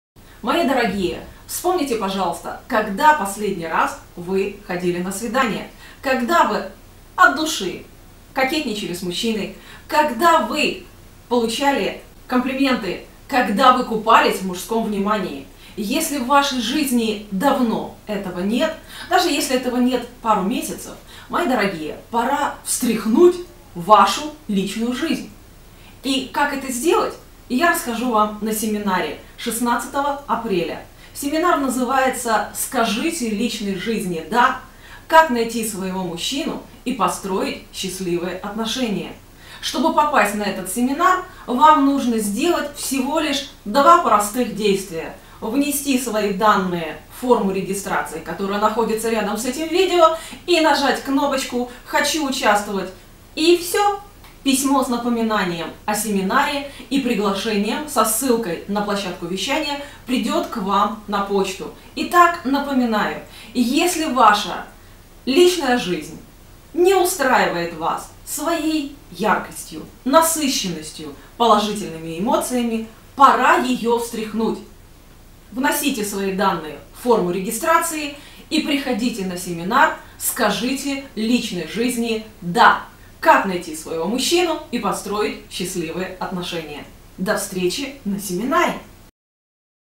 Если нет личной жизни : Что делать, если нет личной жизни у женщины
Бесплатный он-лайн семинар для женщин "Скажите личной жизни - ДА! Как женщине снова начать ходить на свидания, найти своего мужчину"

На семинаре вы узнаете:
    Как начать снова ходить на свидания и распрощаться с одиночеством
    Куда и почему исчезает исчезает личная жизнь из жизни женщины
    Главное условие, при котором она появляется и приносит радость
    Какие конкретные действия избавляют от одиночества
    5 слагаемых счастливой личной жизни
    Какую женщину мужчины с радостью позовут на свидание
    Как стать такой женщиной в самый короткий срок независимо от возраста
    От каких женских поступков зависит судьба счастливых отношений с мужчиной